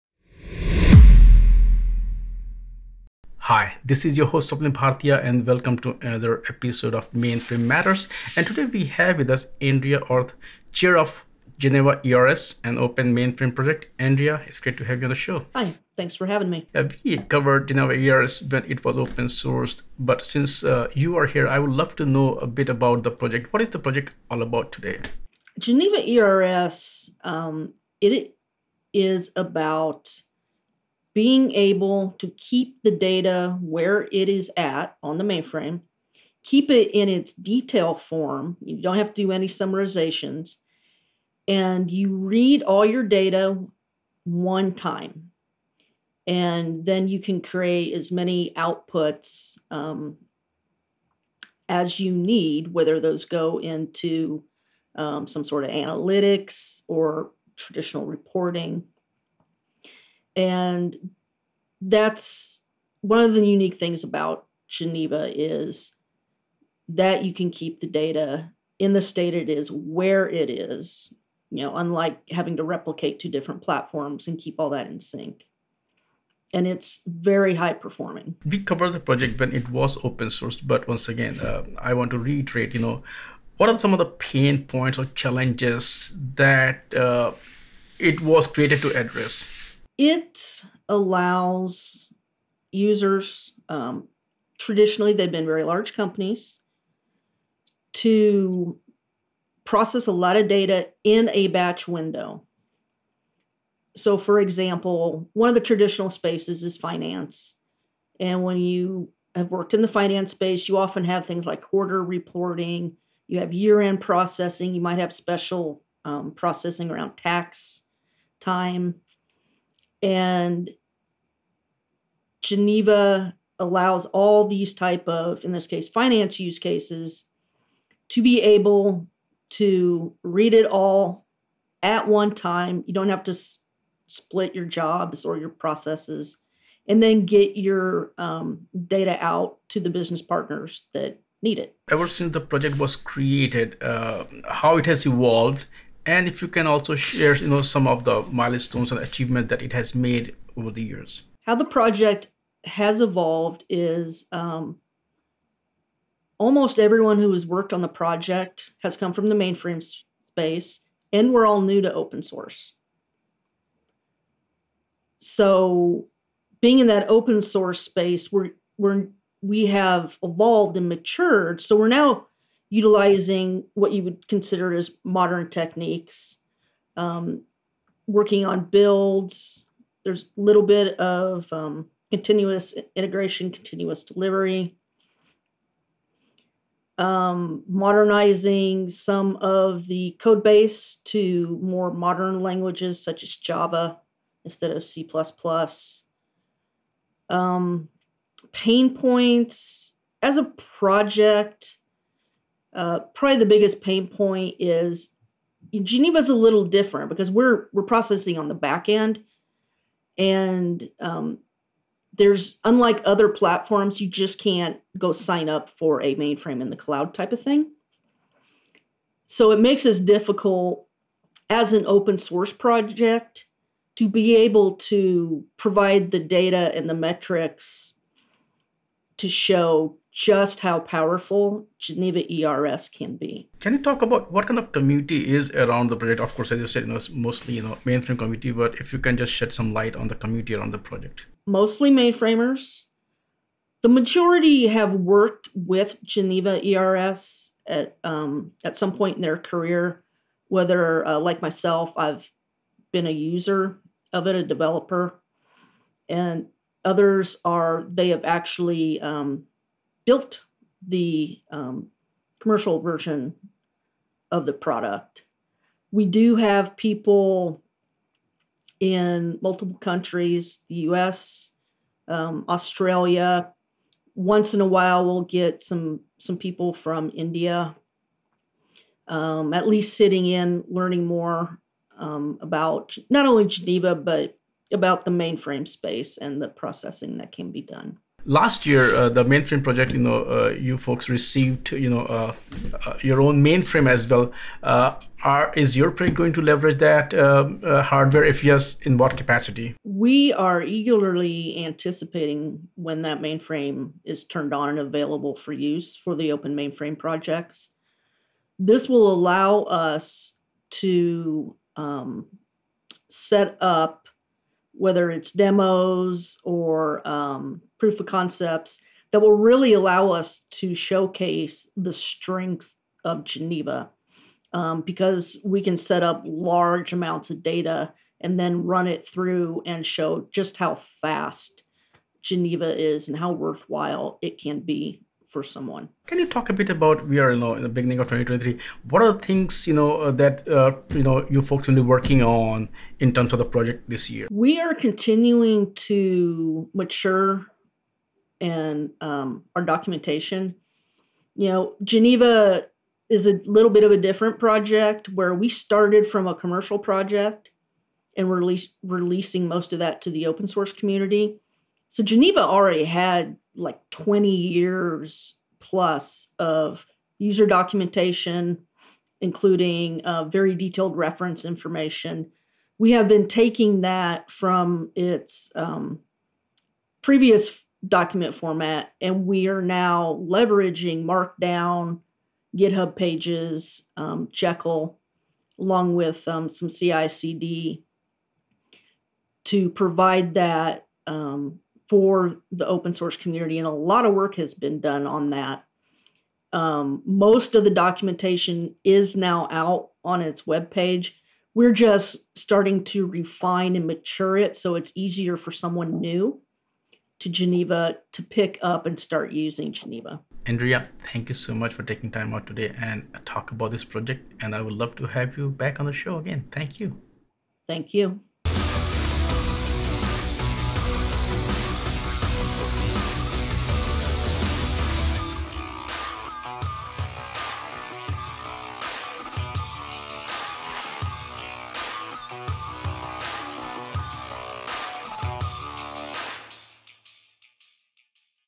GenevaERS: More Than Just A Reporting Solution
Guest: Andrea Orth (LinkedIn)
Project: Open Mainframe Project (Twitter)
The Geneva Enterprise Reporting System was originally developed by PricewaterhouseCoopers and later acquired by IBM as a high-level mainframe reporting solution. Under the Open Mainframe Project (OMP), GenevaERS combines that processing capability of the original version and open-source techniques to become a data extraction and transformation engine specifically tuned for high-volume systems.
In this episode of TFiR Mainframe Matters, Swapnil Bhartiya sits down with Andrea Orth, Chair of GenevaERS at OMP, to talk about the status of the project and the community around it.